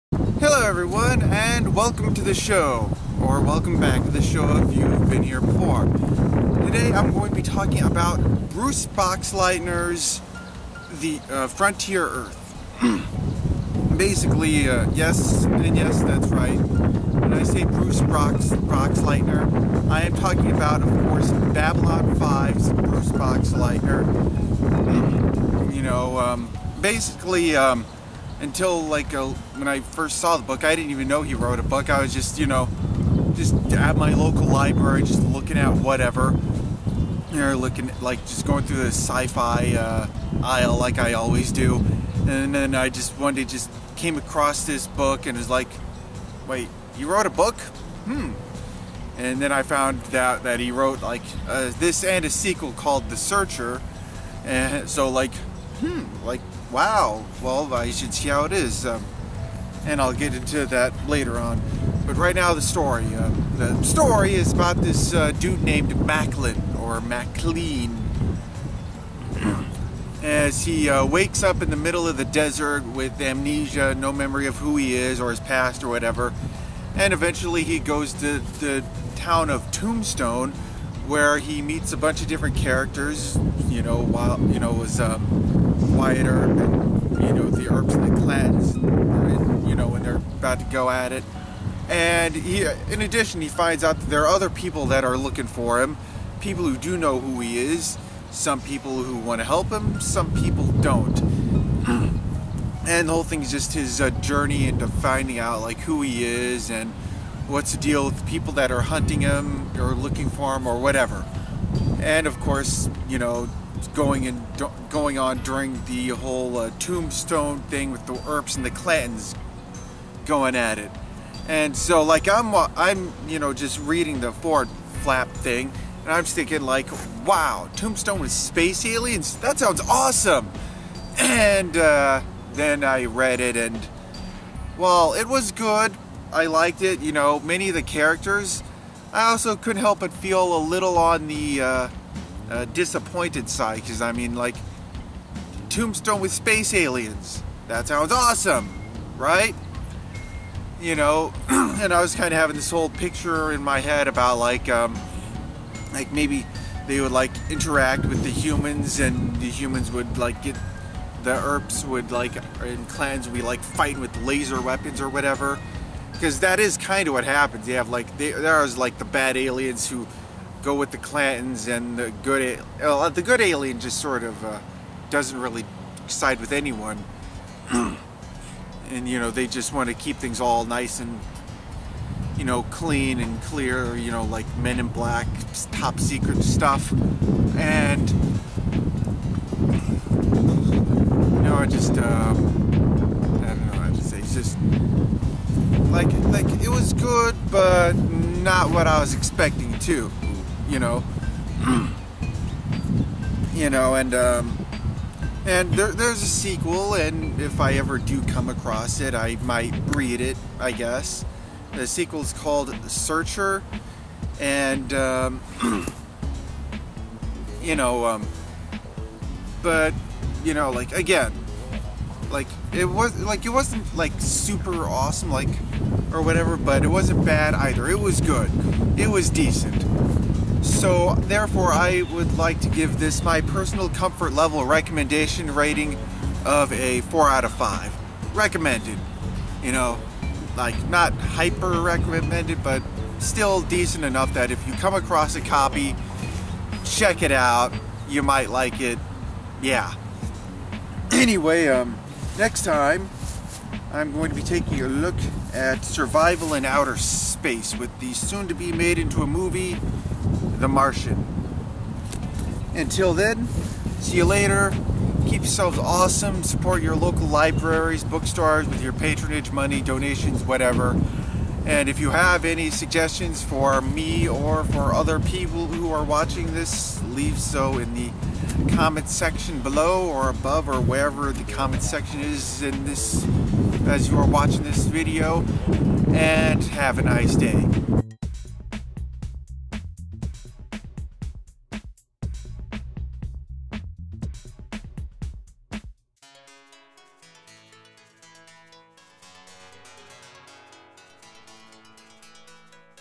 The Biblio files 075 Frontier Earth
Did you know Bruce boxleitner wrote a book?  Me neither.